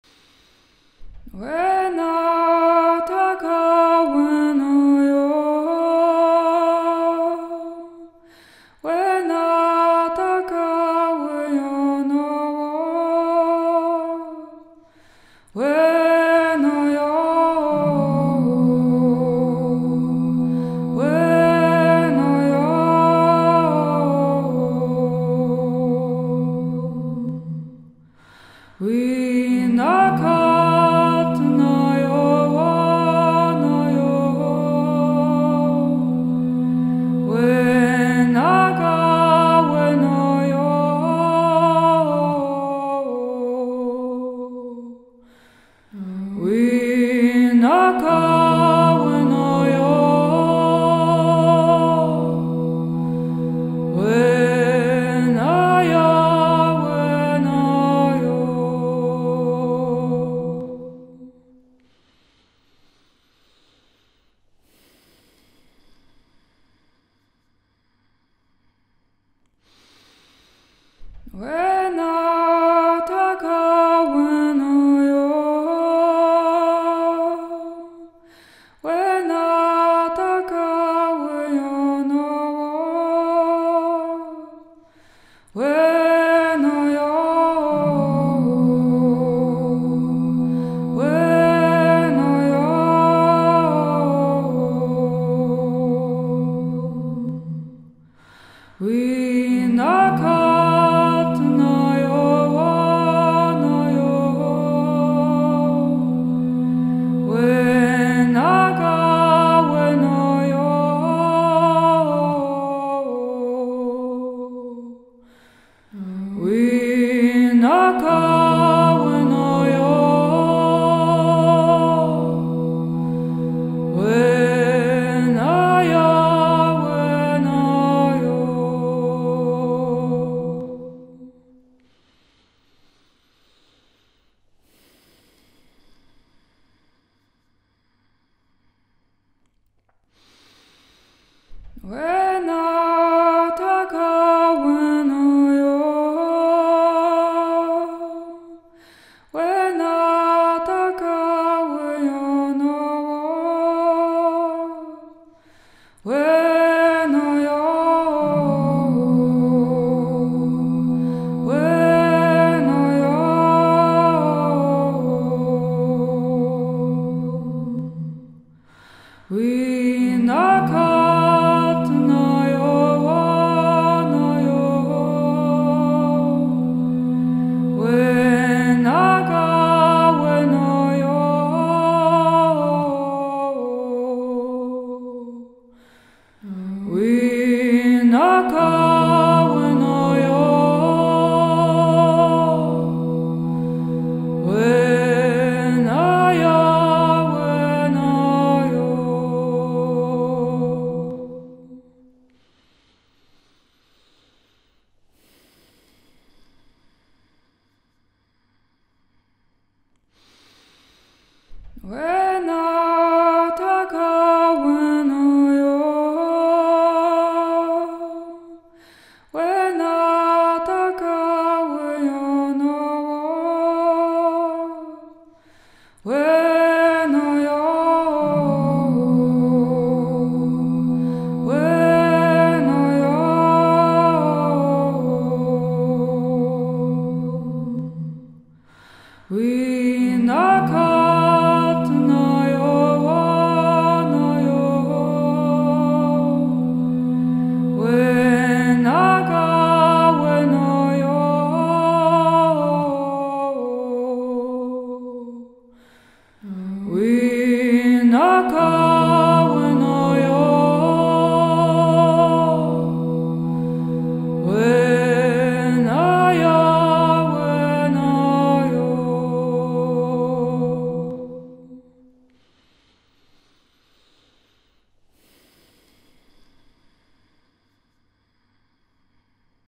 Morning Glory 💚 - Sounds of Healing
Last week I was in the forest very early and I saw the sun rise above the trees. These moments bring me back in alignment and make my heart fill with love and gratitude. There is this mystic vibe in these kind of atmospheres that feels so peaceful and full of glory. Enjoy!